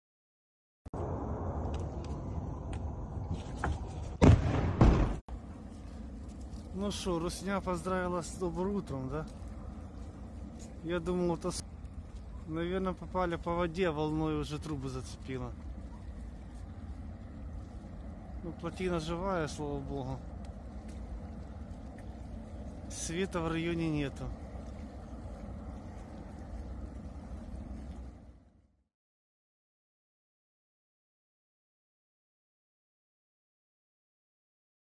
Russian strikes hit Ukraine's largest dam
Ukraine’s state hydropower company said on Friday that a Russia had struck country’s largest dam, the DniproHES in Zaporizhzhia, but that there was no risk of a breach. Footage showed flames and smoke billowing from the dam after Moscow launched its largest assault on Ukraine's energy infrastructure. In an acerbic post this morning responding to Russian attacks targeting energy infrastructure, Volodymyr Zelenskiy, the Ukrainian president, said “Russian missiles have no delays, unlike aid packages for Ukraine.”